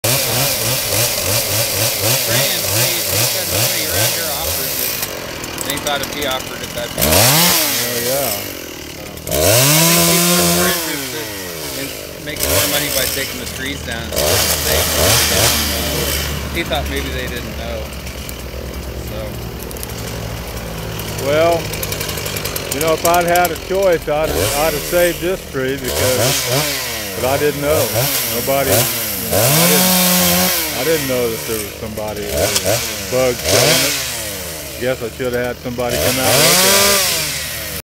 Removing a dead Pine Tree that has been infected with the mountain pine beetle
Removing a dead Pine Tree, this Pine  tree died from the infestation of the mountain pine beetle commonly found in Colorado, the mountian pine beetle has moved east into Kansas, There is a huge number of pine trees infected and Killed by the Mountian Pine Beetle, It takes about a year after the Mountain Pine Beetle infects a Pine tree before the tree dies.